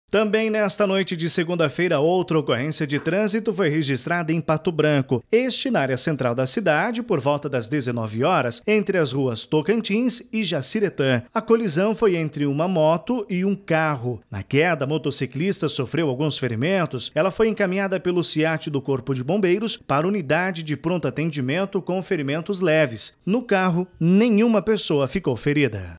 Motociclista fica ferido após acidente de trânsito
Segurança: O acidente foi registrado na noite desta segunda-feira (18) na área central de Pato Branco.